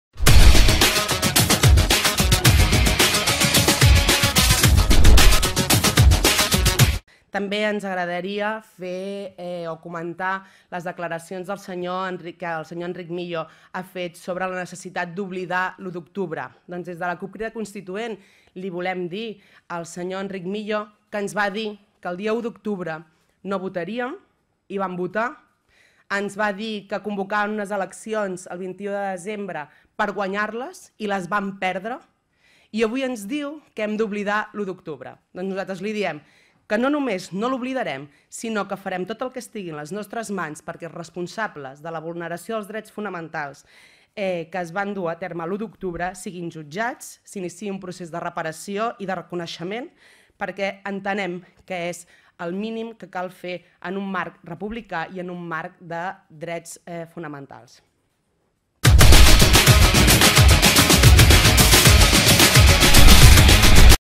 CUP Sr Millo NO Olvidamos el 1 Octubre
Rueda de Prensa que realiza que realiza la CUP en el Parlament en que da respuesta a Enric Millo en las declaraciones que hizo a la televisión 8tv en las que dijo que habría que olvidar y pasar pagina al 1 de Octubre, la respuesta de la CUP es que no olvidaran. Fuente Canal Parlament.